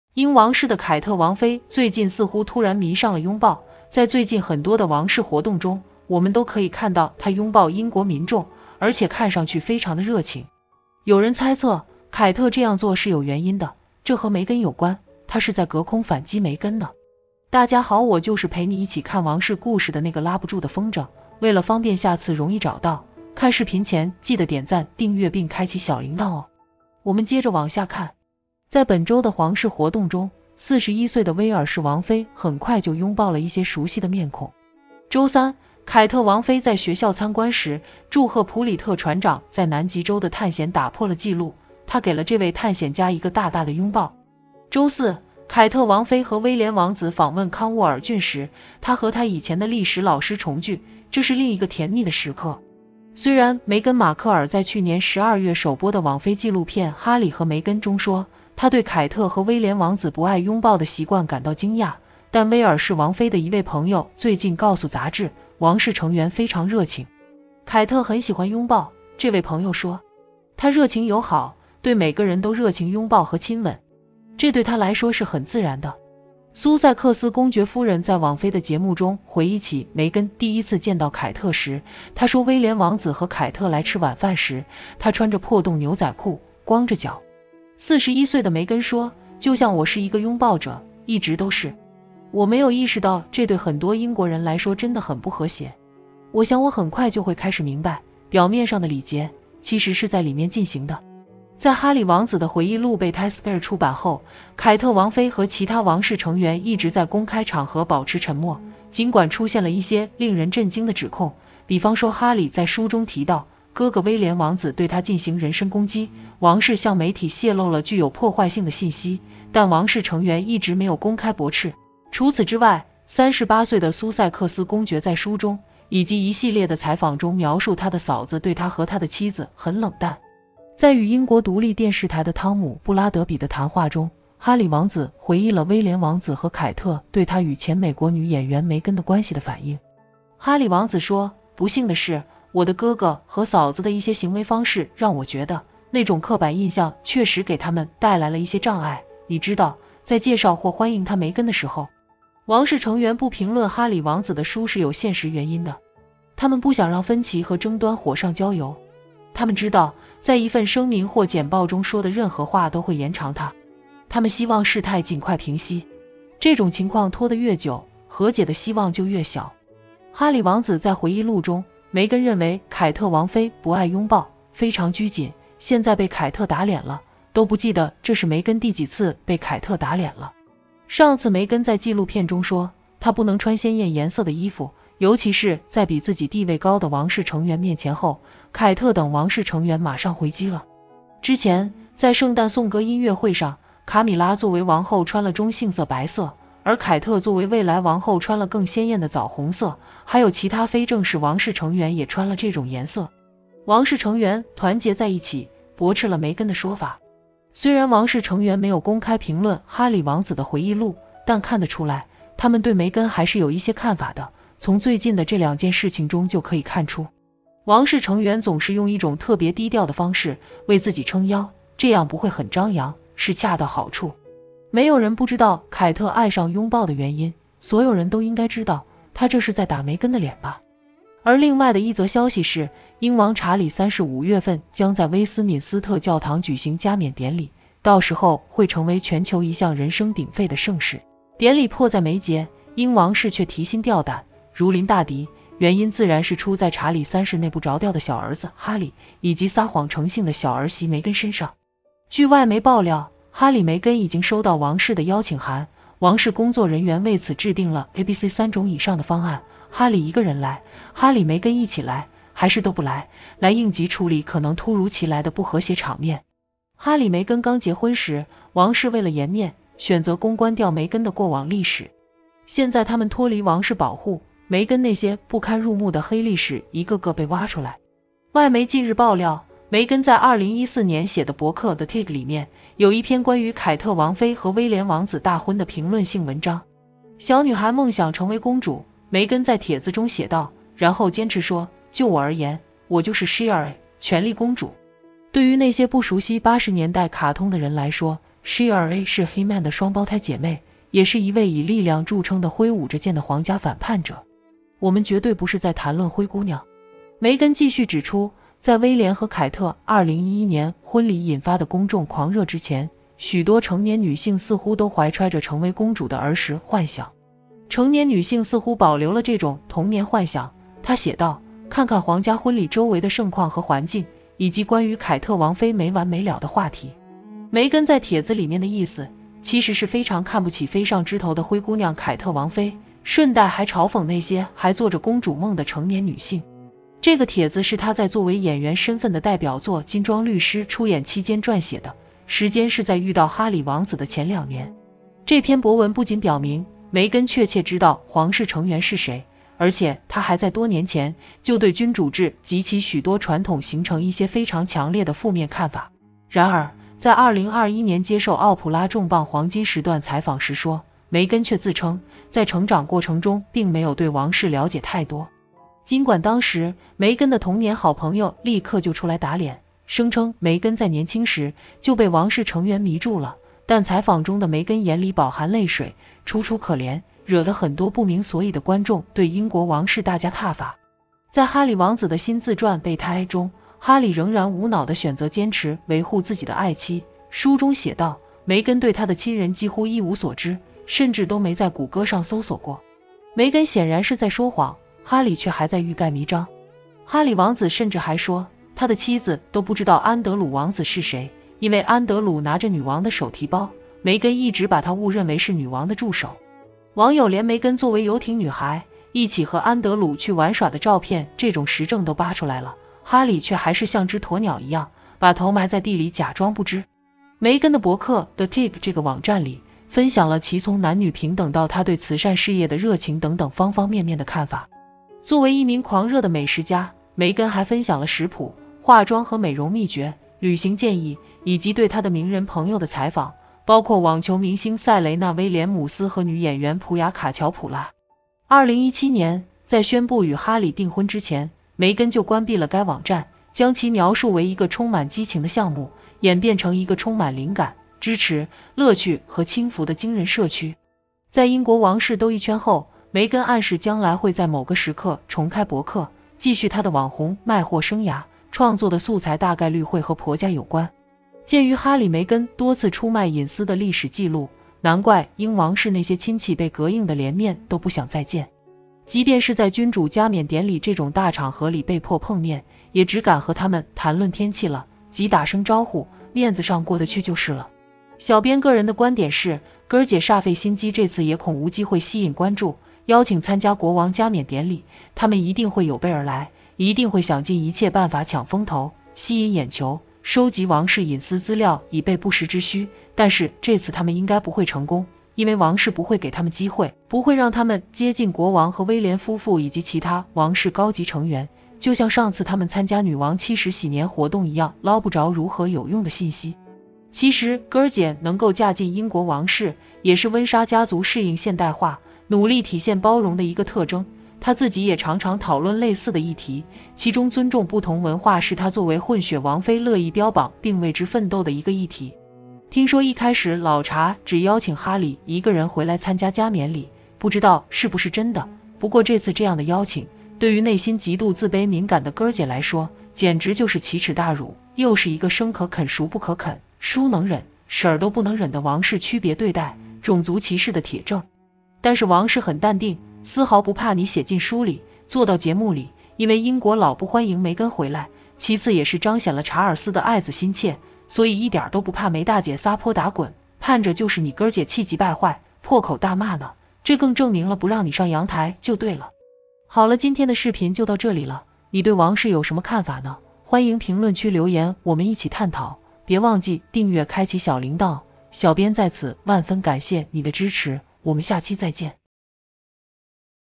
凱特突然愛上了擁抱！疑似是對梅根隔空反擊：又被打臉，梅根撒謊被抓現行！英王室制定三種方案歡迎參加典禮
凱特突然愛上了擁抱！疑似是對梅根隔空反擊：又被打臉，梅根撒謊被抓現行！英王室制定三種方案歡迎參加典禮#凱特王妃#梅根#哈里王子#威廉王子#戴安娜#夏洛特公主#路易斯#乔治